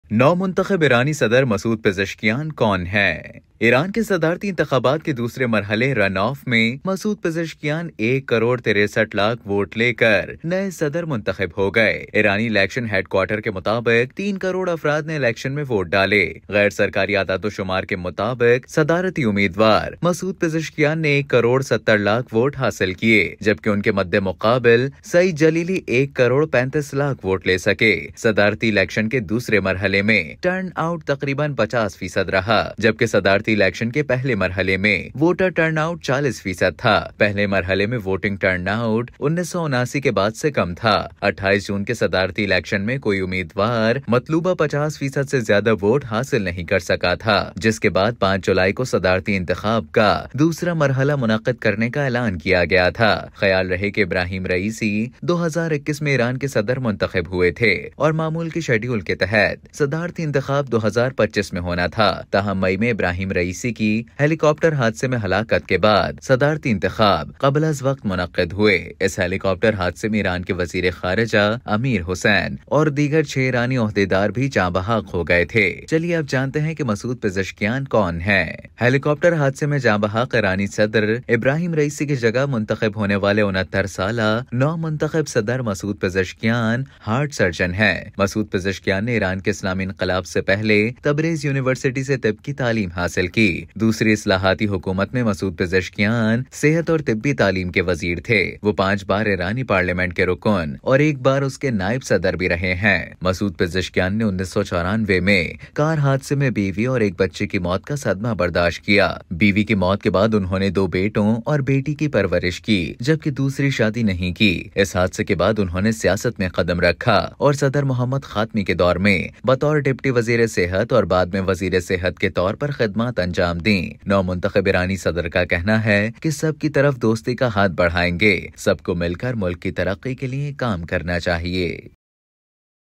نومنتخب ایرانی صدر مسعود پزشکیان کون ہیں؟ | Masoud Pezeshkian | Iran
Masoud Pezeshkian elected as the new President of Iran in the runoff election, securing 16.3 million votes.